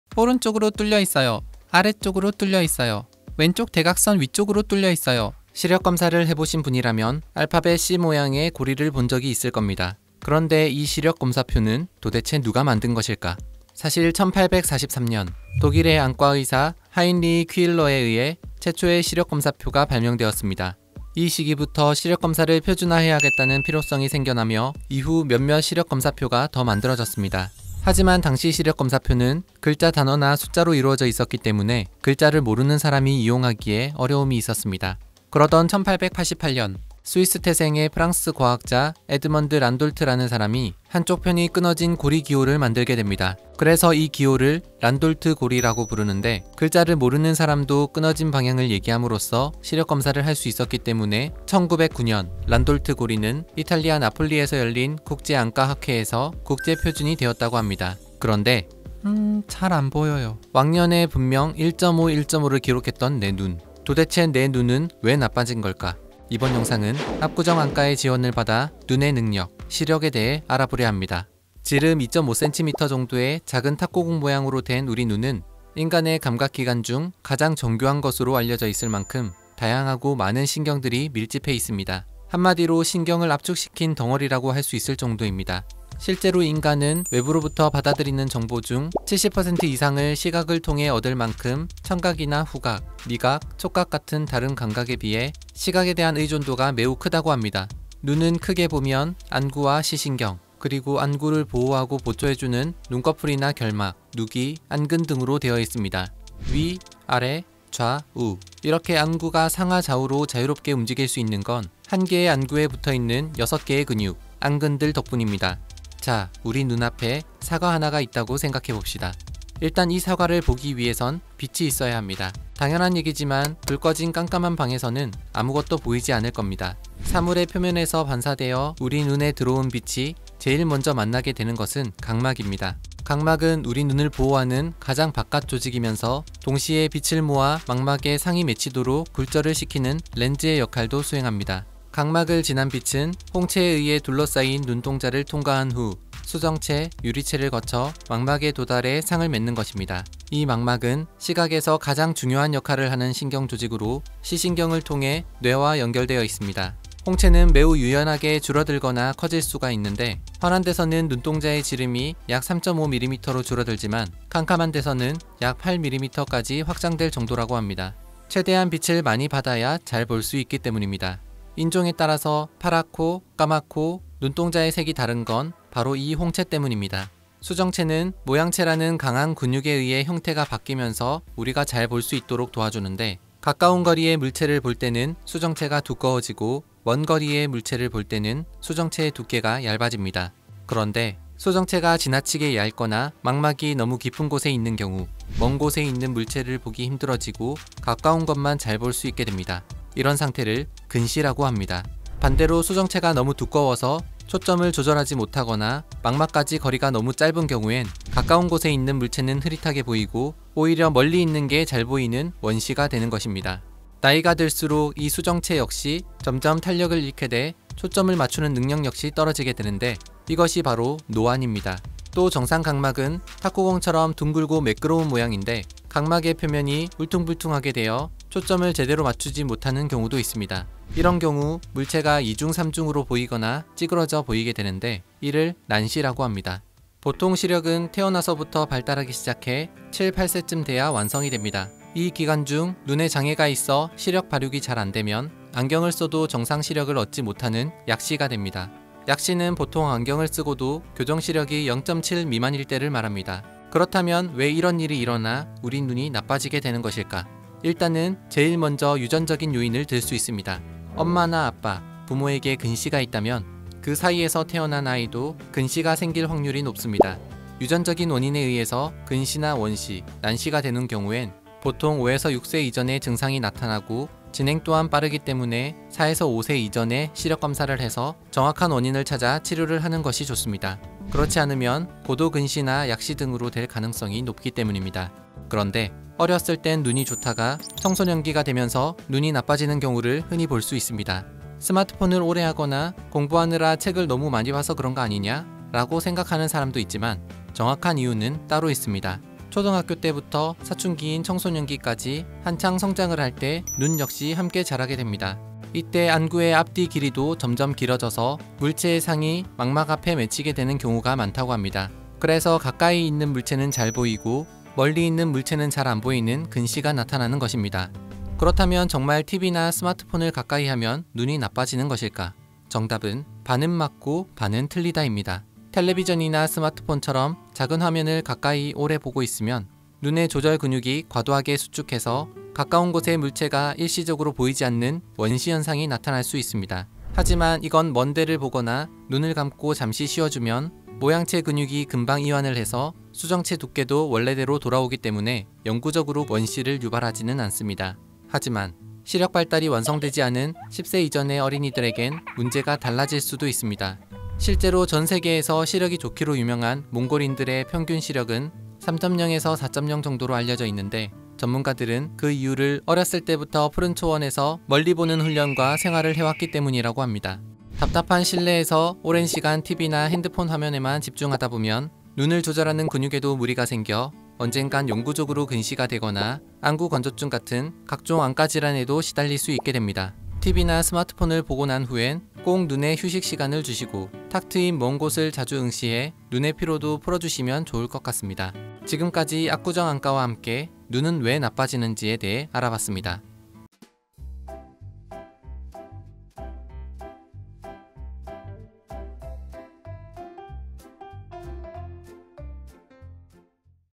눈이 나빠지는 이유 [건강백과사전]
대부분의 사람들은 살면서 적어도 한 번은 시력검사를 한다.
그런데, 저 시력검사표는 도대체 누가 만들었을까? 왜 만들었을까?
그나저나 1.5, 1.5 건강하던 내 눈은 왜 나빠진걸까?
눈의 구조와 눈이 나빠지는 이유에 대해 알아보자.

안녕하세요 세상의 모든 지식입니다 (^ ^)
본 영상은 압구정안과와 함께 제작하였습니다.

우리 모두 지식인이 되는 그날까지 열심히 영상 만들겠습니다.